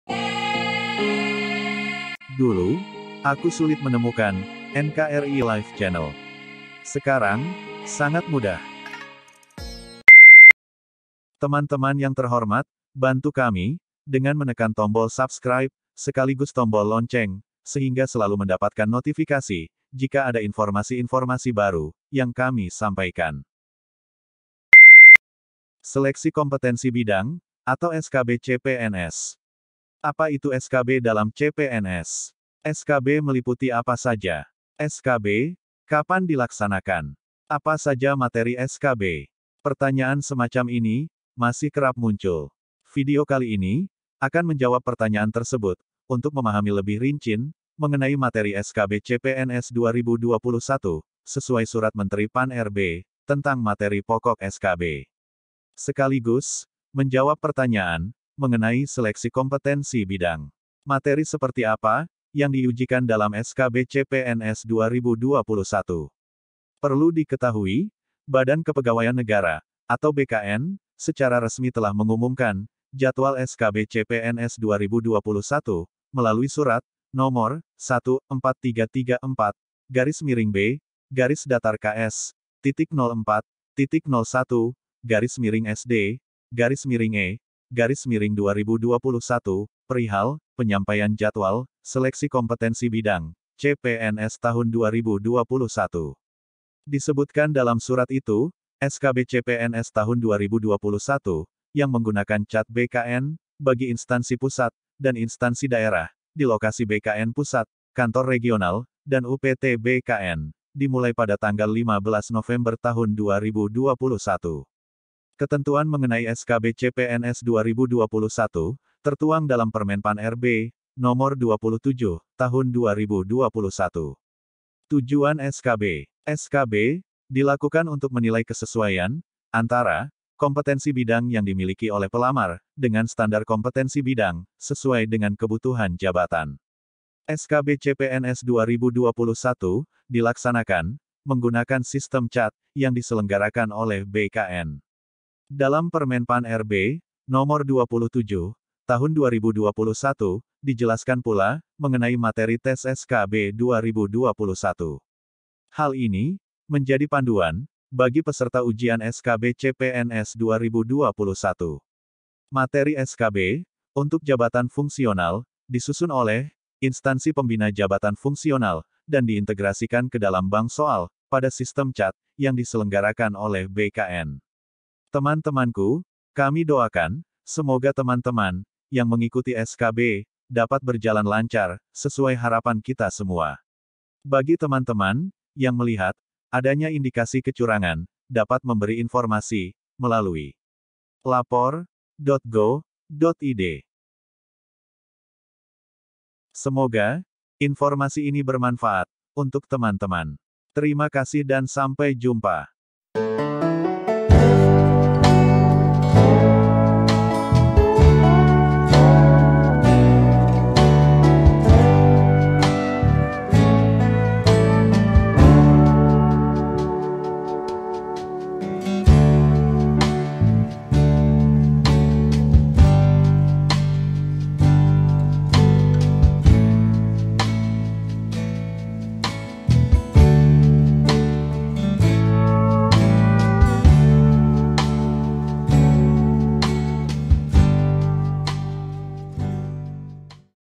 Apa itu SKB dalam CPNS? | SKB meliputi apa saja? | SKB, kapan dilaksanakan? | Apa saja materi SKB ?
Seleksi Kompetensi Bidang, atau SKB CPNS

Apa itu SKB dalam CPNS? SKB meliputi apa saja? SKB,  kapan dilaksanakan?
Apa saja materi SKB ?

Pertanyaan semacam ini, masih kerap muncul

Video kali ini, akan menjawab pertanyaan tersebut, untuk memahami lebih rinci, mengenai materi SKB CPNS 2021, sesuai Surat Menteri PAN RB, tentang materi pokok SKB

Sekaligus, menjawab pertanyaan, mengenai seleksi kompetensi bidang

Materi seperti apa, yang diiujikan dalam SKB CPNS 2021

Perlu diketahui,  Badan Kepegawaian Negara, atau BKN, secara resmi telah mengumumkan, jadwal SKB CPNS 2021, melalui surat, Nomor: 14334/B-KS.04.01/SD/E/2021, perihal, Penyampaian Jadwal, Seleksi Kompetensi Bidang, CPNS Tahun 2021

Disebutkan dalam surat itu, SKB CPNS Tahun 2021, yang menggunakan CAT BKN, bagi Instansi Pusat, dan Instansi Daerah, di lokasi BKN Pusat, Kantor Regional, dan UPT BKN, dimulai pada tanggal 15 November 2021

Ketentuan mengenai SKB CPNS 2021, tertuang dalam Permenpan RB, Nomor 27, Tahun 2021

TUJUAN SKB

SKB, dilakukan untuk menilai kesesuaian, antara, kompetensi bidang yang dimiliki oleh pelamar,, dengan standar kompetensi bidang, sesuai dengan kebutuhan Jabatan

SKB CPNS 2021, dilaksanakan, menggunakan sistem CAT,  yang diselenggarakan oleh BKN

Dalam Permenpan RB, Nomor 27, Tahun 2021, dijelaskan pula, mengenai materi tes SKB 2021

Hal ini, menjadi panduan, bagi peserta ujian SKB CPNS 2021

Materi SKB, untuk Jabatan Fungsional, disusun oleh, instansi pembina Jabatan Fungsional, dan diintegrasikan ke dalam bank soal, pada sistem CAT, yang diselenggarakan oleh BKN.

teman temanku, kami doakan, semoga teman teman, yang mengikuti SKB, dapat berjalan lancar, sesuai harapan kita semua

bagi teman teman, yang melihat, adanya indikasi kecurangan, dapat memberi informasi, melalui :

lapor.go.id

Terima Kasih 🙏🙏